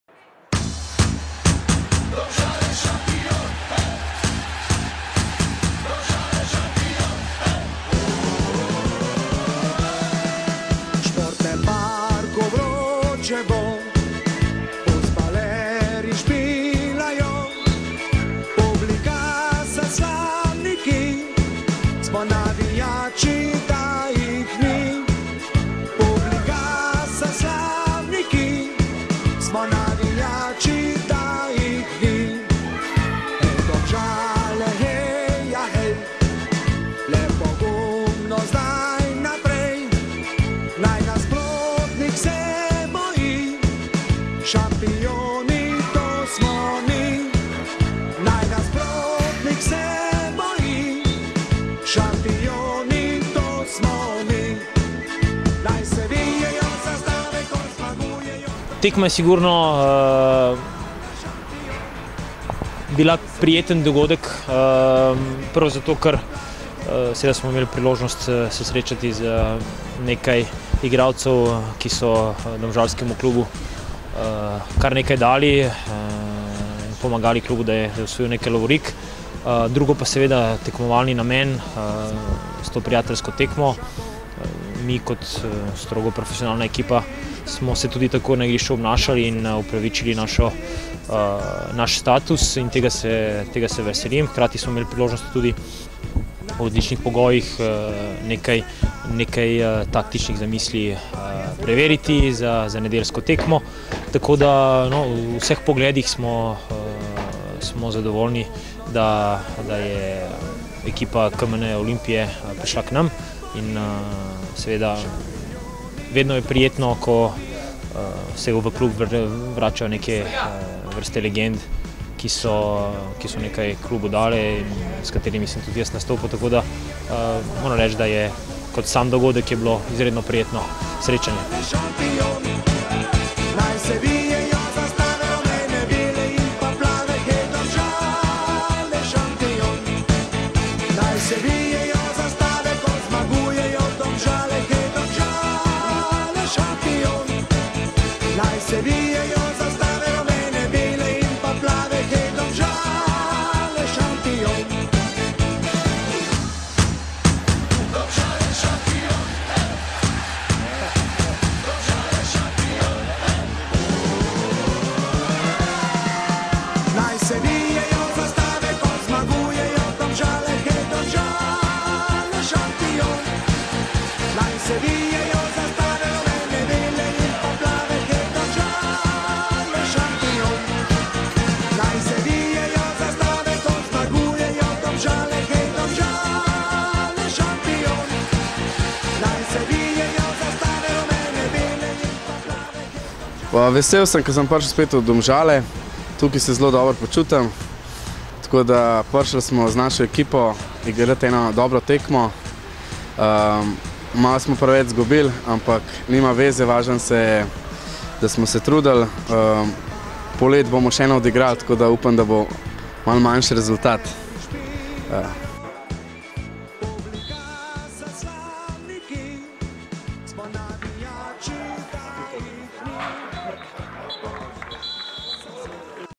Vrhunci prijateljske tekme Domžale - KMN Olimpija 7:0
Nogometaši Domžal so se v torek zvečer v zanimivem prijateljskem obračunu pomerili z moštvom KMN Olimpija, v katerem v večini igrajo nekdanji slovenski reprezentanti in člani prvoligaških klubov. V Športnem parku v Domžalah so tako v udarni enajsterici zaigrali Sebastjan Cimerotić, Ermin Raković, Darijan Matić, Dalibor Teinović, Igor Lazić, Selvad Kujović, Ljubiša Štrbac, Erik Salkić, Suvad Grabus, Miroslav Grbić in vratar Domen Božjak. Imenitna ekipa, ki je na trenutke pokazala izjemno tehnično znanje, toda na koncu so prevladale mladost, fizična pripravljenost in taktična disciplina članskega moštva Nogometnega kluba Domžale, ki je slavilo z visokim izidom 7:0.